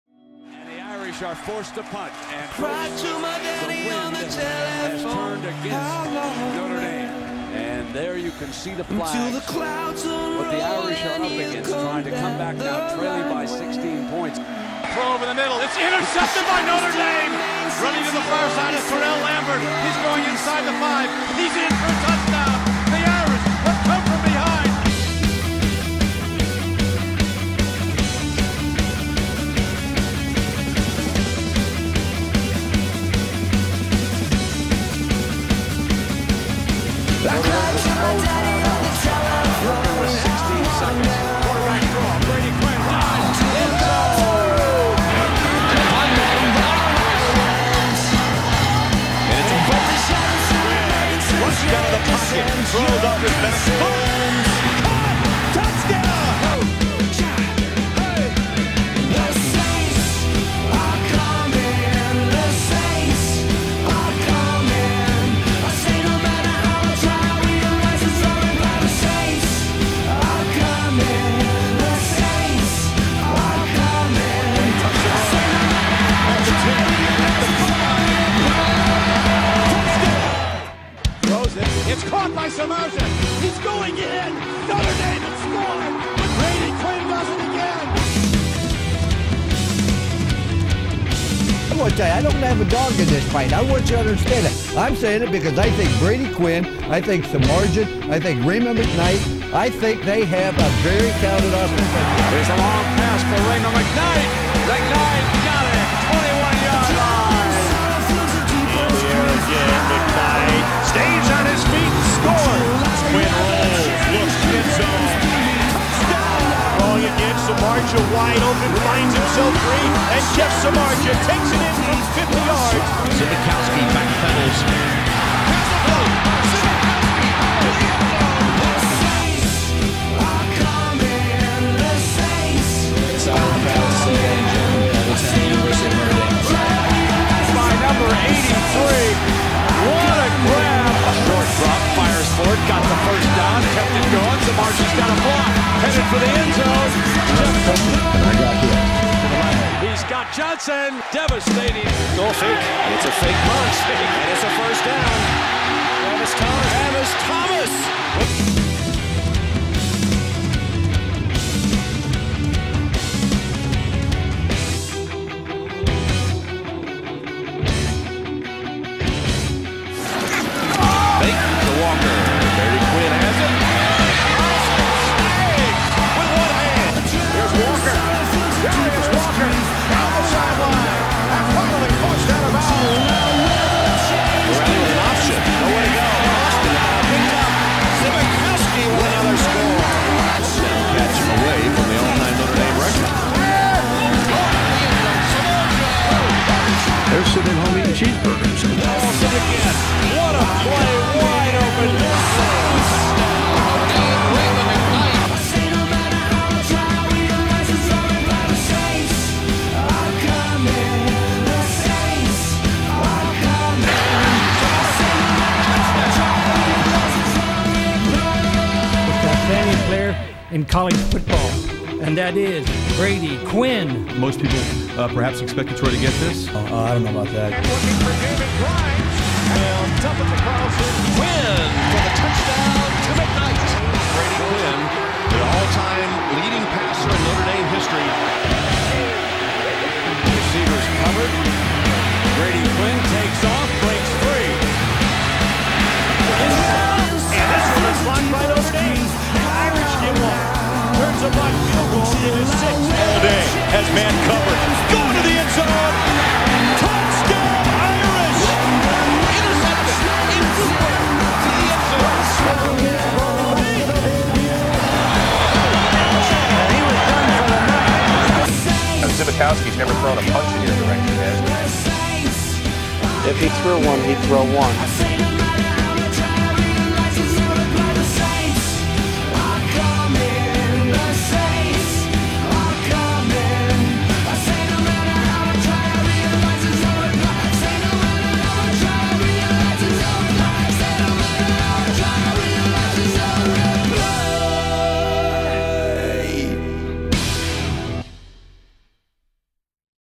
2006 Notre Dame Highlights by Oldie
Highlights of Notre Dame Football's 2006 season